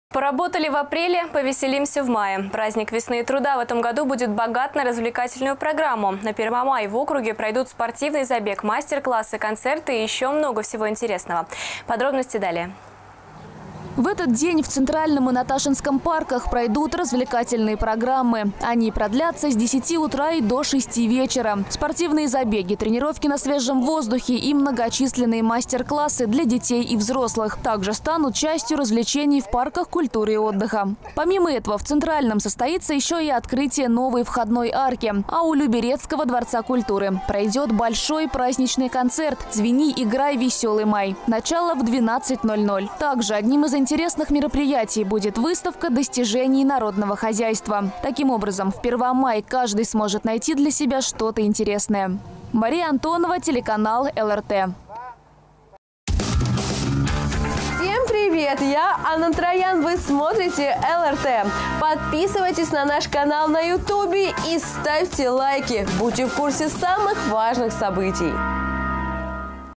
Концерт и открытие паркового сезона что ждет люберчан на Первомай
Первомай в этом году будет богат на развлекательную программу. С самого утра в округе начнутся мероприятия: спортивный забег, мастер-классы, концерты и еще много всего интересного. Подробнее о том, что вас ждет, далее.

В этот день в Центральном и Наташинском парках пройдут развлекательные программы. Они продлятся с 10:00 утра и до 18:00 вечера. Спортивные забеги, тренировки на свежем воздухе, и многочисленные мастер-классы для детей и взрослых также станут частью развлечений в парках культуры и отдыха. Помимо этого, в Центральном состоится еще и открытие новой входной арки. А у люберецкого Дворца культуры пройдет большой праздничный концерт «Звени, играй, веселый май». Начало в 12:00. Также одним из интересных мероприятий будет выставка достижений народного хозяйства. Таким образом, в Первомай каждый сможет найти для себя что-то интересное.

Мария Антонова